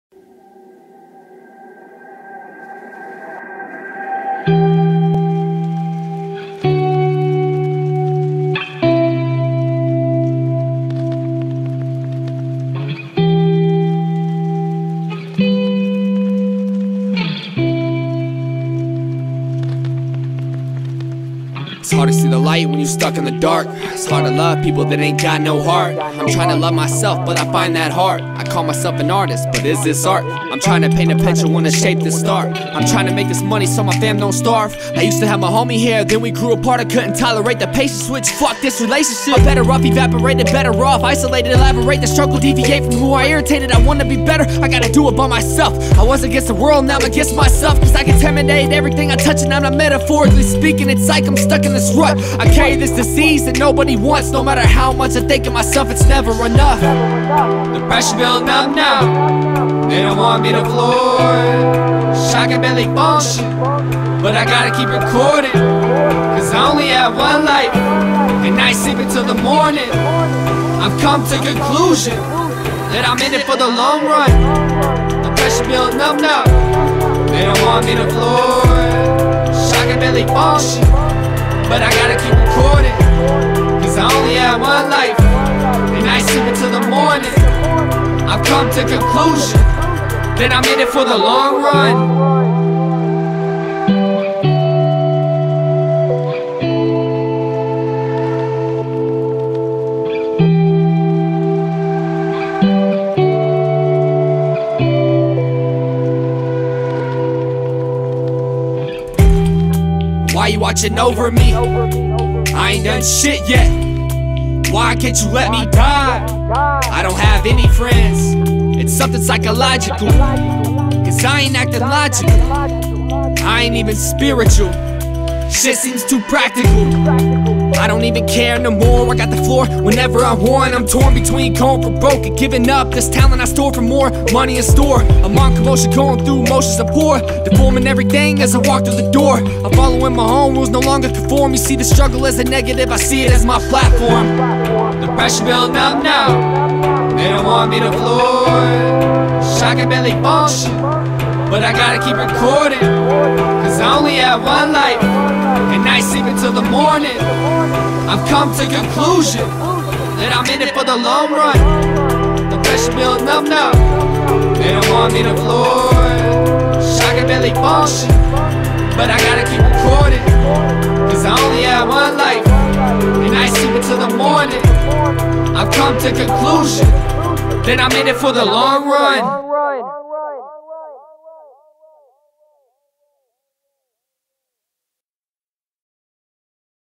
Nikon D810 Music Video (sample)
My first music video using the Nikon D810 & Nikon 24-120mm f4 lens.